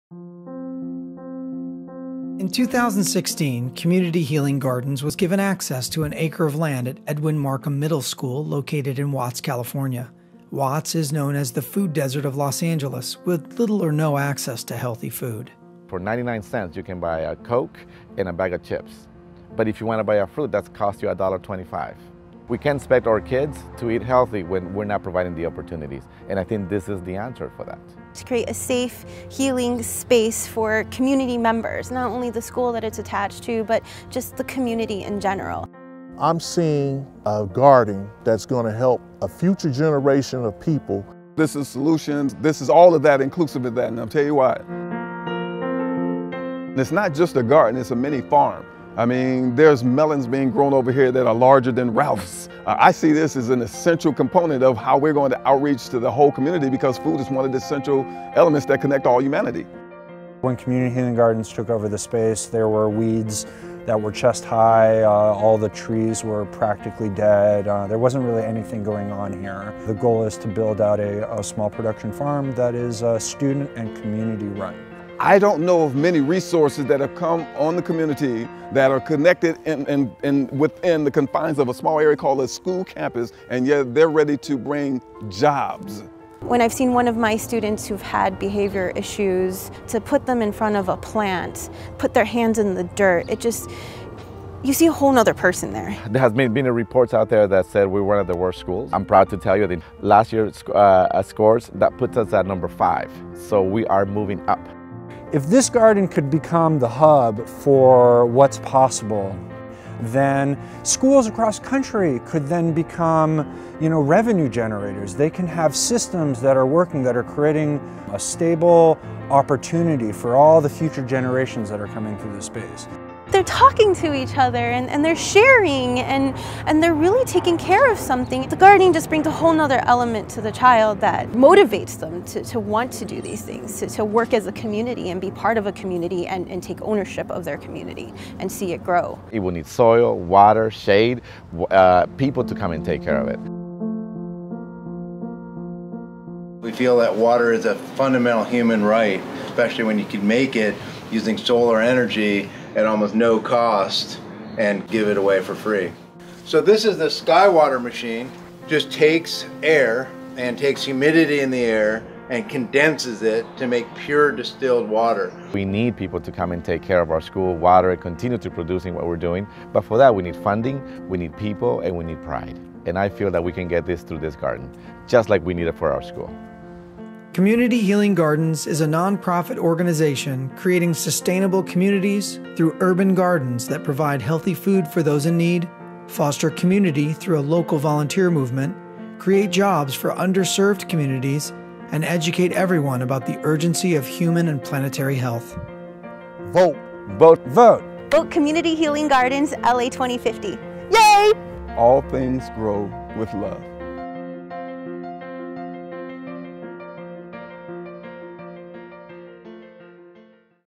Watts Healing Garden by Community Healing Gardens
Community Healing Gardens (CHG) is building a working urban garden called the “Watts Healing Garden” on a one-acre piece of land at Edwin Markham Middle School in Watts, CA. Also known as the “food desert” of Los Angeles, Watts suffers from one of the highest rate of diabetes and heart disease in Los Angeles.

In collaboration with Markham Middle School, CHG is embarking on a three-year project to create a sustainable, working farm that provides healthy food for the community, an educational space for the school, and a safe place to promote mental, physical, and human and planetary healing. Additionally, the garden will provide much needed jobs as well as an educational center for the community.

Our motto - All things grow with LOVE.

Video: That Step Productions